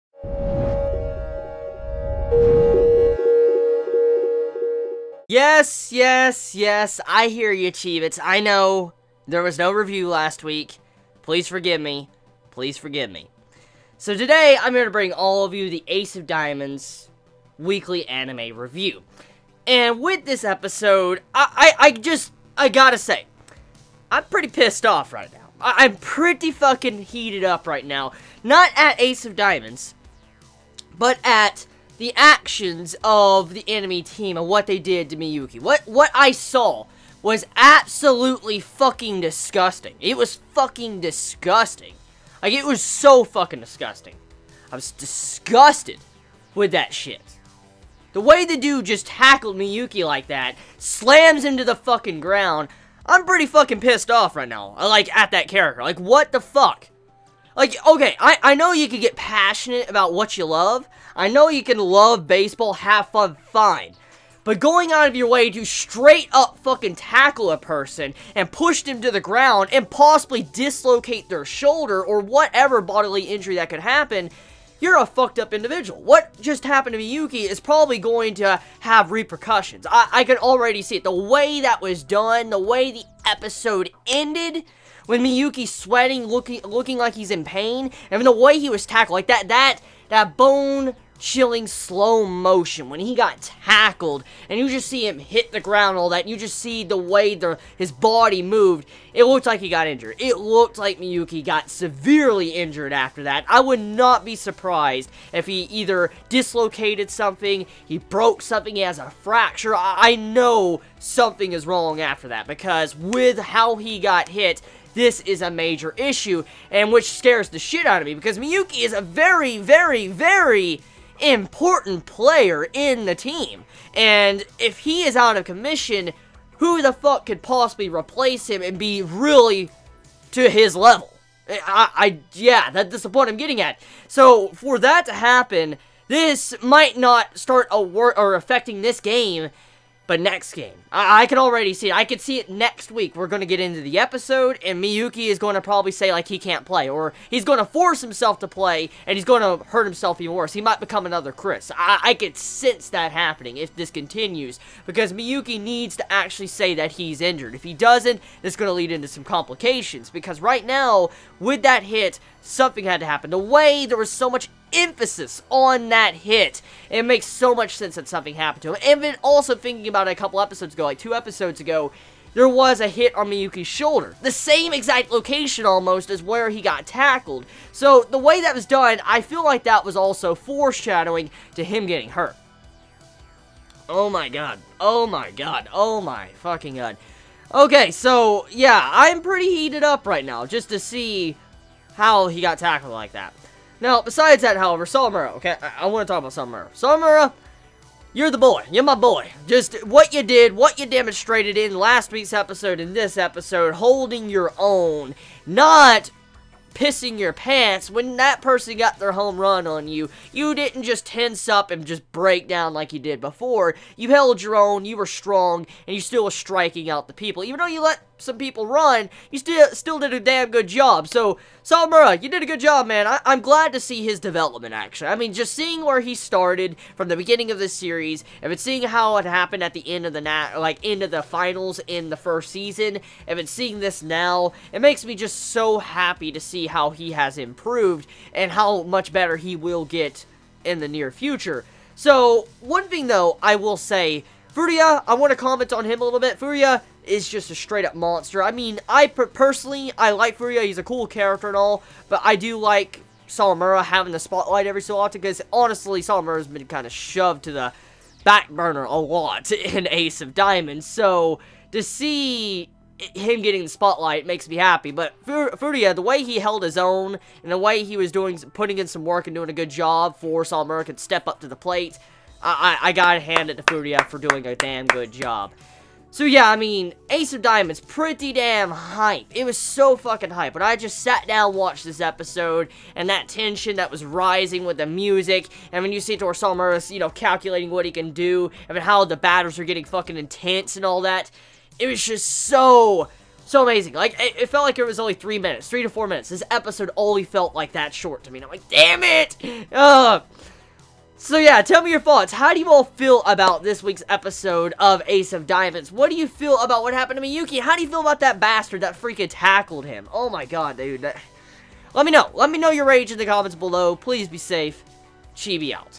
Ace of Diamond Season 2 Episode 34 Anime Review - Miyuki Nooooo
Ace of Diamond Season 2 Episode 34 puts Miyuki in a situation that might be very bad in the near future. Ready for Diamond no Ace S2 Episode 35 Review?

For any fellow Chibit that wants to send something:
2069 Johnson Chapel Road, 38583, Sparta, TN